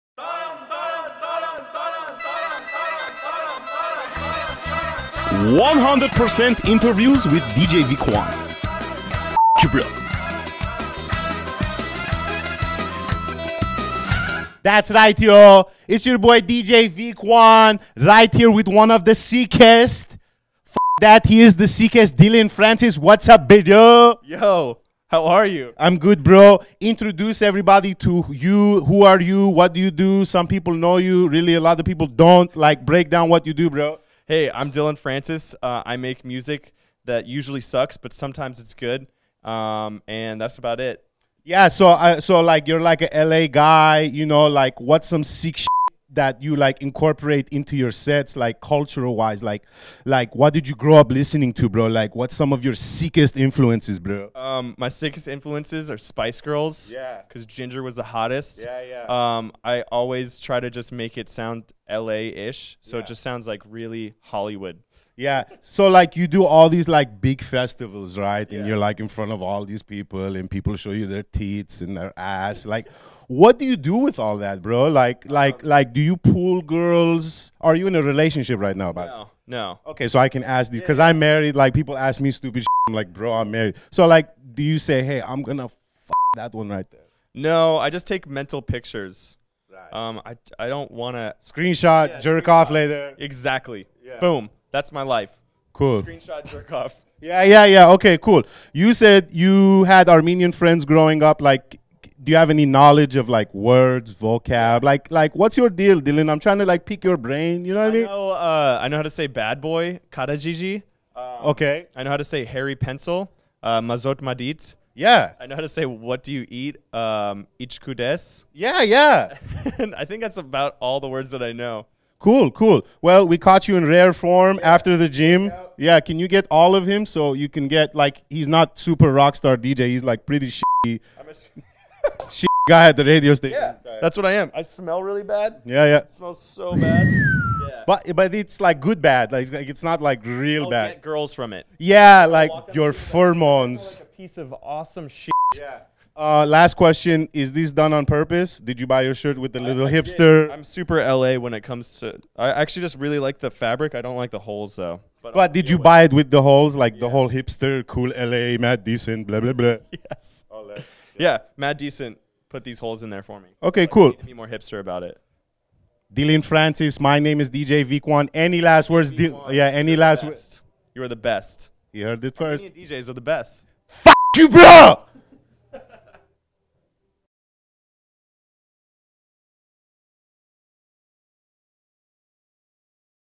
Dillon Francis Is Really Hollywood | 100% Interviews w/ Dj Vick One
Dillon Francis stopped by Power 106 and spoke with VickOne about his influences, The Spice Girls, having a hipster shirt and his Hollywood music,